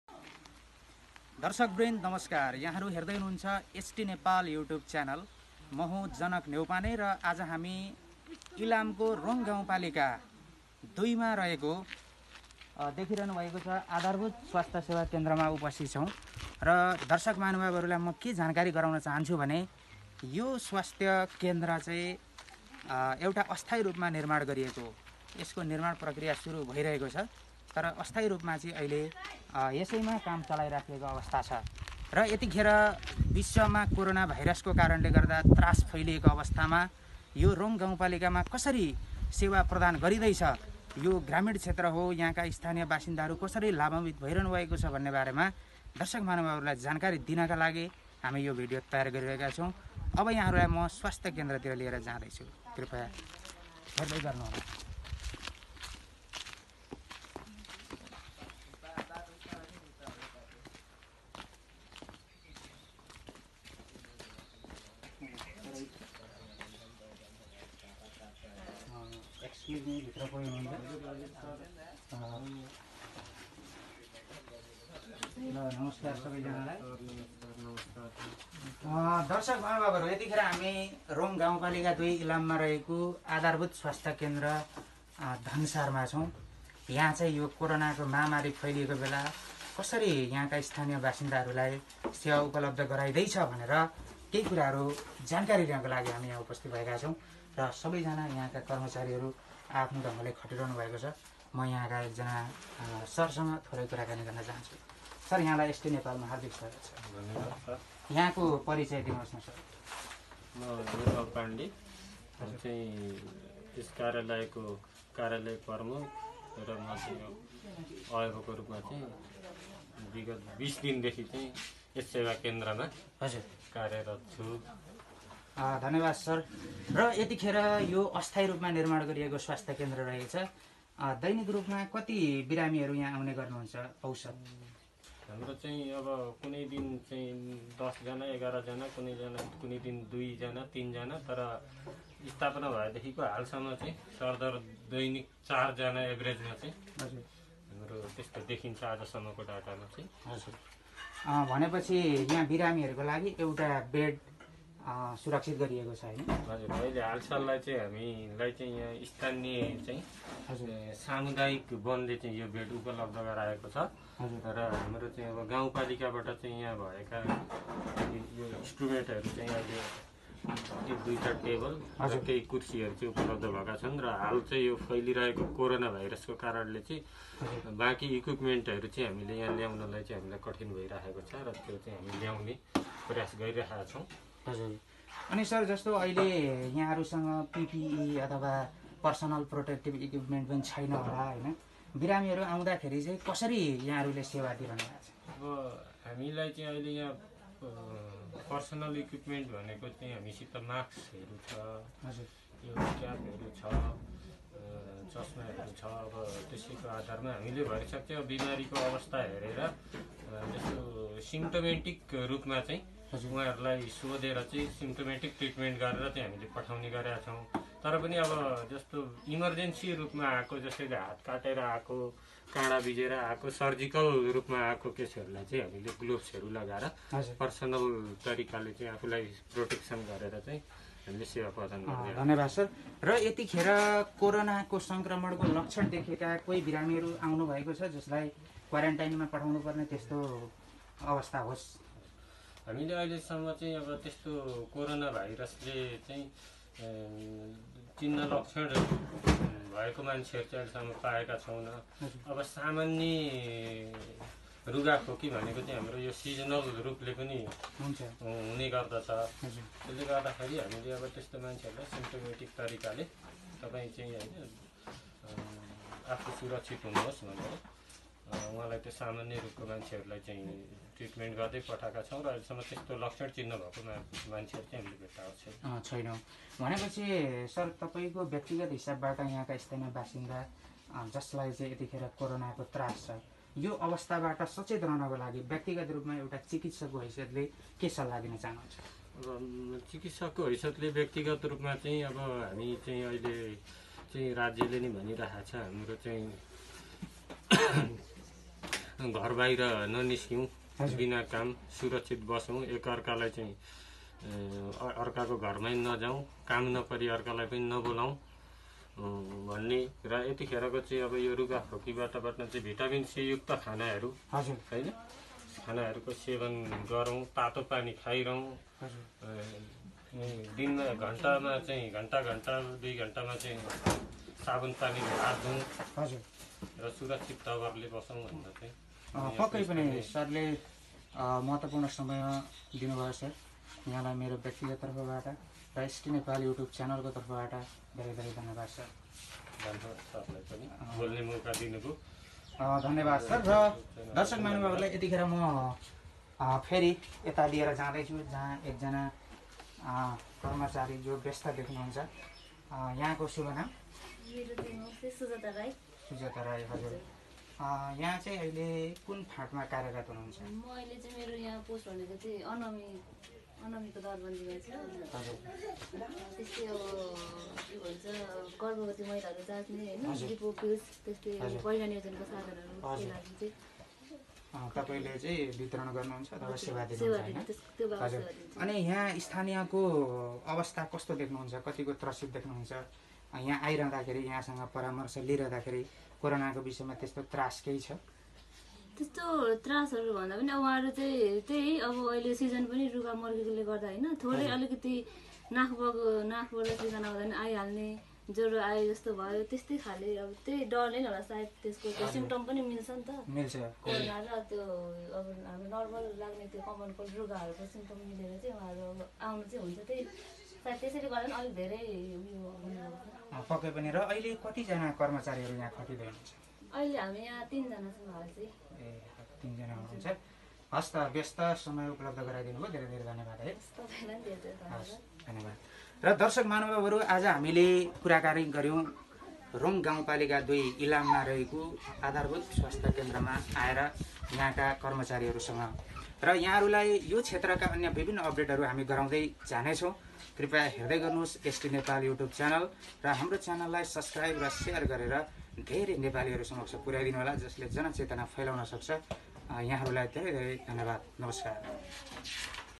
इलाम मा ( कोरोना) लक डाउन को प्रभाव | Lockdown effect on Nepalese people | ilam | with Janak
Health condition of Nepalese people